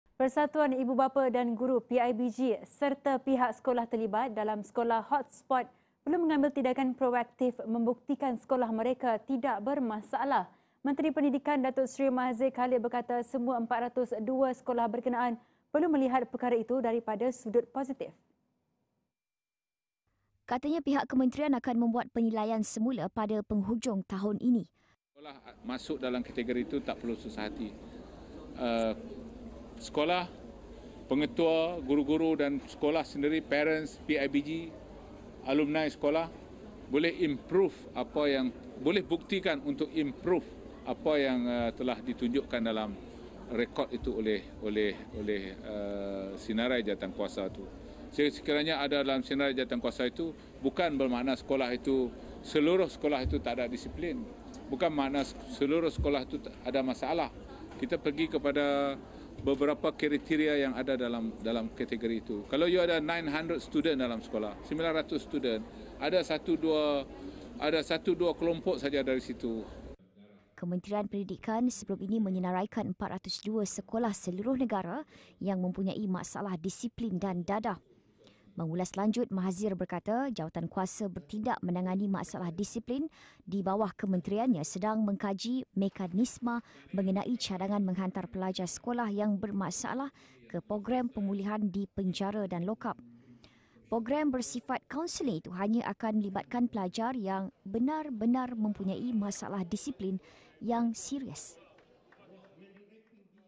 PIBG dan sekolah perlu ambil tindakan proaktif
Persatuan Ibu Bapa dan Guru (PIBG) serta pihak sekolah terlibat dalam sekolah hotspot perlu mengambil tindakan proaktif membuktikan sekolah mereka tidak bermasalah.